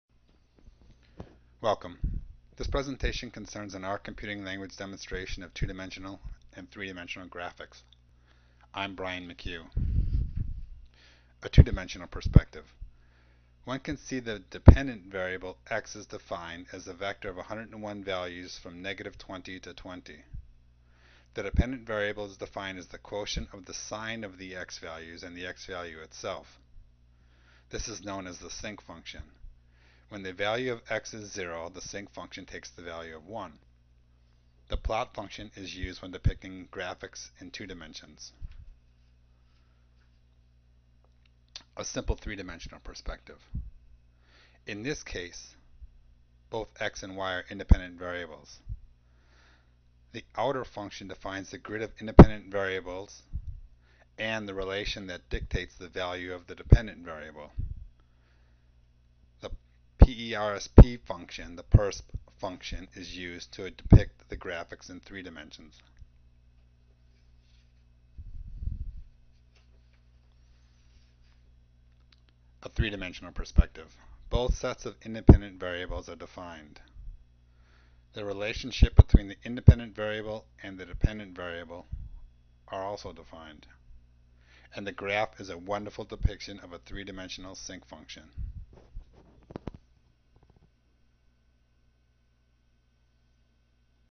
12/15/2008 - 2D and 3D graphics with the R computing language
As presented by bjmchugh to the HVRSD Educational Program Committee of the BoE.